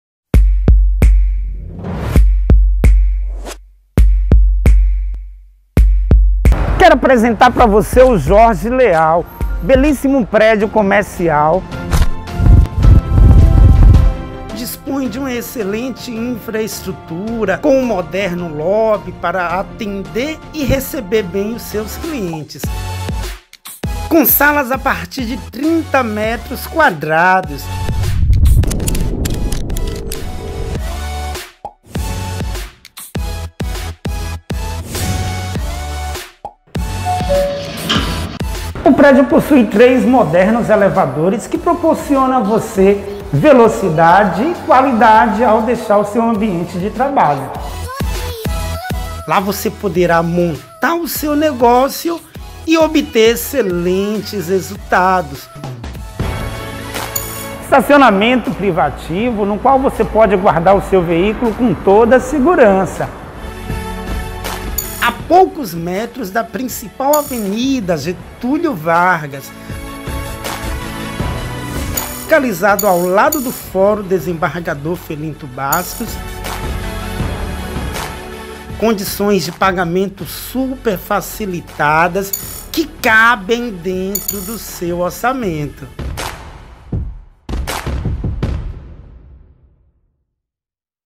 Jorge Leal Trade Center - Feira de Santana
O moderno edifício empresarial Jorge Leal Trade Center é o empreendimento certo para investidores, empreendedores e advogados. Com excelente localização, situado próximo ao fórum Felinto Bastos, possui escritórios à altura do seu sucesso e uma estrutura inédita para sua empresa.

Esse belo edifício composto por 132 Salas, 9 lojas e 1 academia; 8 pavimentos tipo com 16 salas por andar; Lobby com pé-direito duplo; Recepção com controle eletrônico de acesso (catracas); Mezanino com 4 salas de reunião, abertas para locação com renda revertida para o condomínio; 3 elevadores; Academia no último andar com varanda e 108m².

Se você está a procura do melhor investimento, o sucesso e o futuro da sua empresa, venha para o Jorge Leal Trade Center e garanta seu espaço.

Consulte as unidades disponíveis e mais informações conosco.